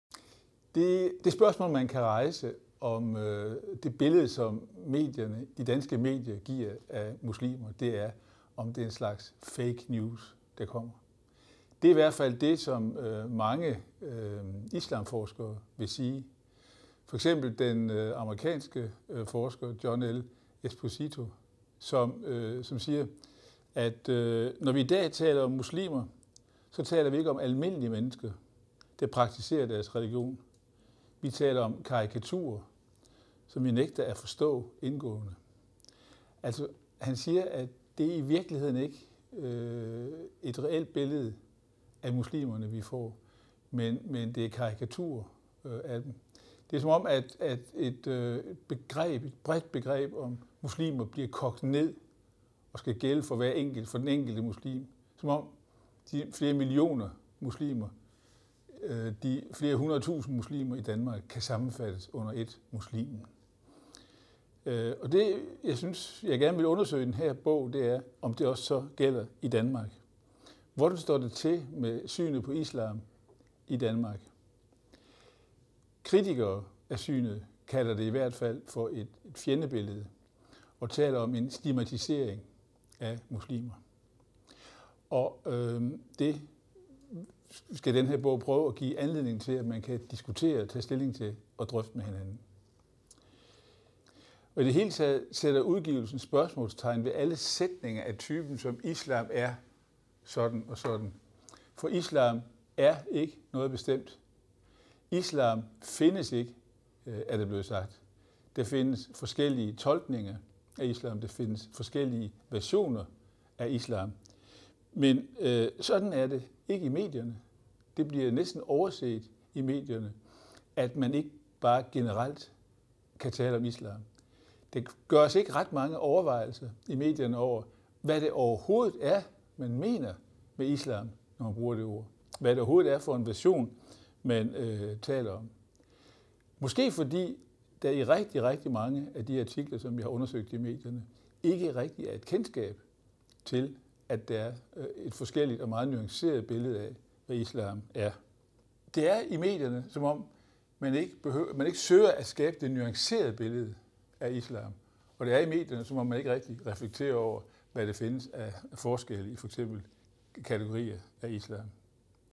Systimeforfatter Preben Brock Jacobsen om Islam i medierne
Hør Systimeforfatter Preben Brock Jacobsen fortælle om sin udgivelse, Islam i medierne.

Fokus i bogen er det billede af islam, der tegnes i de danske aviser.

Islam i medierne gennemgår 10 positioner i kritik og forsvar af islam, fx:

– Er islam kvindefrigørende eller kvindeundertrykkende?
– Skaber islam terror og rædsel, eller misbruger terrorister islam?
– Kan islam forenes med demokrati og menneskerettigheder?

Udgivelsen indledes med en kort introduktion til islam og gennemgår desuden klart og overskueligt de forskellige versioner af islam. Hertil kommer klare introduktioner til diskurs- og argumentationsanalyse, herunder en illustrativ oversigt over argumentationstyper.

Udgivelsen lægger op til elevernes selvstændige arbejde med kvalitativ og kvantitativ metode. Desuden introducerer Preben Brock Jacobsen hvordan en diskursanalyse kan bruges i religionsfaget.